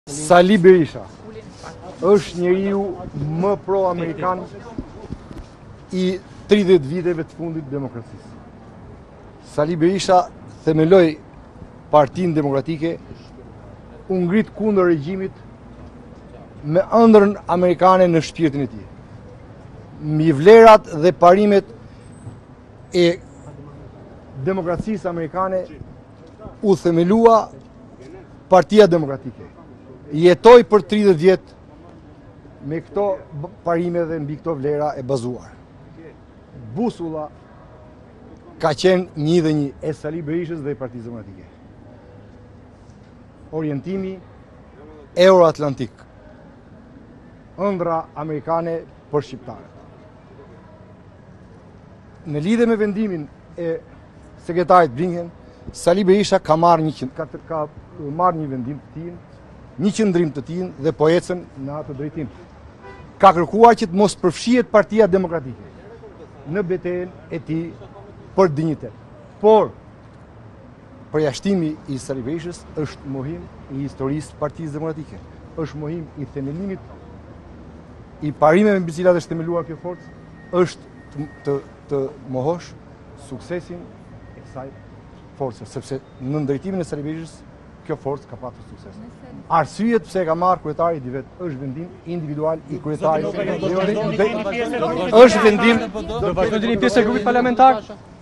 Përjashtimi i Berishës, Noka: Vendim personal i Bashës, është mohim i historisë së PD-së
Deputeti Flamur Noka ka folur publikisht kundër vendimit të Lulzim Bashës për përjashtimin e Sali Berishës nga grupi parlamentar. Noka tha se vendimi është individual i Bashës dhe se më këtë vendim ka mohuar historinë e PD-së.

'Sali Berisha është njëriu më pro amerikan i 30 viteve të fundit të demokracisë, Sali Berisha themeloi PD dhe u ngfrit kundër regjimit me ëndrrën amerikanë në shpirtin e tij. mbi vlerat dhe parimet e Amerikës u themelua PD. Busulla ka qenë një dhe në e Sali Berishës dhe PD-së orientimi euro atlantik. Sali Bersiha ka mbajtur një qendrim të tij dhe ka kërkuar që PD të mos përfshihem. Është mohim i histrorisë së PD-së, i parimit të kësaj forcë është të mohosh suksesin e kësaj forcë. Arsyet i di kryetari është vendimi i kryetarit është vendim individual.'- tha Noka.